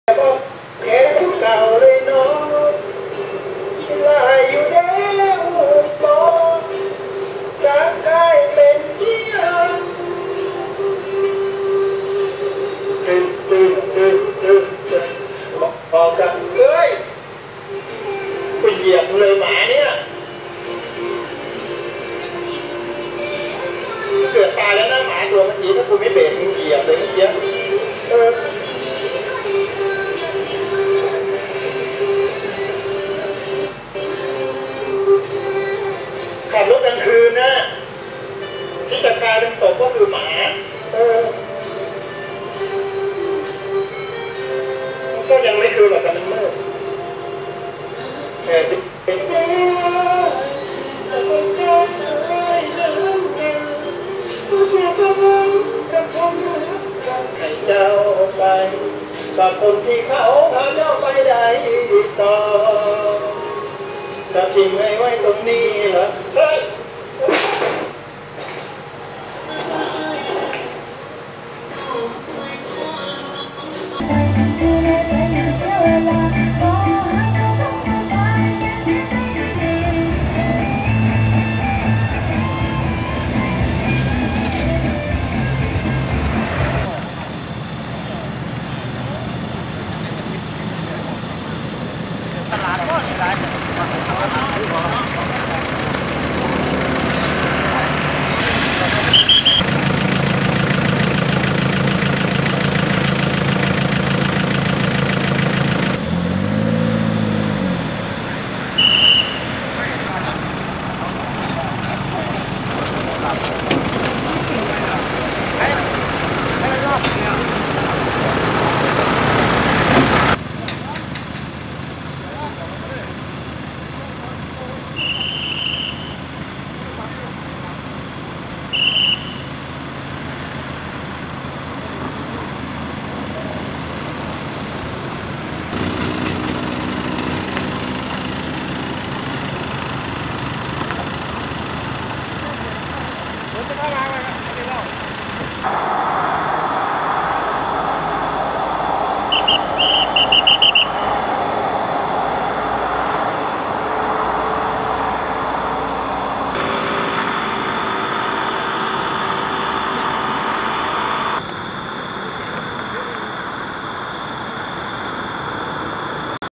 อุบัติเหตุไลฟ์สดร้องเพลงขณะขับขี่ l เรื่องจริงผ่านเฟส
อุบัติเหตุ สี่แยกวัดใจ !!! รถเก๋งไลฟ์สดร้องเพลงขณะขับขี่ ก่อนเกิดเหตุเกือบชนสุนัข แต่สุดท้ายมาเจ็บที่ สี่แยกวัดใจ โครมผ่านออนไลน์
ตามประเด็นปมการเกิดอุบัติเหตุ สี่แยกวัดใจ ที่ผ่านมารถ เก๋งชนกับรถกระบะ มีผู้ได้รับบาดเจ็บ 1 รายเป็นชาย บริเวณสี่แยกศรีสำโรง ตำบลแคน อำเภอสนม จังหวัดสุรินทร์ ในจุดเกิดเหตุนี้เป็นแยกศรีสำโรง ต.แคน มักเหตุอุบัติเหตุบ่อยครั้ง เป็นถนนสี่เลนไม่มีไฟแดง ชาวบ้านจึงเรียกแยกนี้ว่า สี่แยกวัดใจ เพราะต้องเดาใจอีกคันเพื่อผ่านแยกนี้ ซึ่งแยกหรือถนนเส้นนี้เป็นถนนหลักที่ต้องใช้ความเร็ว แม้จะมีป้ายบอกทางแยกแต่ก็ไม่ได้ช่วยลดการเกิดอุบัติเหตุ ชาวบ้านหรือทางเทศบาลตำบลแคนเคยยื่นเรื่องขอติดตั้งไฟแดงจากหน่วยงานที่รับผิดชอบไปหลายครั้งแล้ว แต่ก็ไม่ได้รับการตอบรับกลับแต่อย่างไร ทำให้เกิดปัญหาการเกิดอุบัติเหตุตรงจุดดังกล่าวบ่อยครั้ง
ต่อมา ทราบขณะรถเก๋งขับขี่มีการไลฟ์สดผ่าน facebook เปิดเพลงร้องเพลงไปด้วยอารมณ์สุนทรีย์ ช่วงไกล้เกิดอุบัติเหตุนั้นผู้ขับขี่รถเก๋ง พูดขึ้นว่า โอ้ยๆ เดียวกรูเหยียบเลยหมาเนี้ย เกือบตายเลยนะหมาตัวเมื่อกี้ ถ้ากรูไม่เหยียบเบรคมรึงเหยียบเลยนะเมื่อกี้ และก็พูดต่อว่า ขับรถกลางคืนนะ ที่จะกลายเป็นศพก็คือหมา ก็ไม่กลางคืนหรอกแต่มันมืด จากนั้นก็ร้องเพลงต่อ สักพักเสียงอุทาน โอ้ยยย..ก่อนสิ้นสุดไลฟ์สด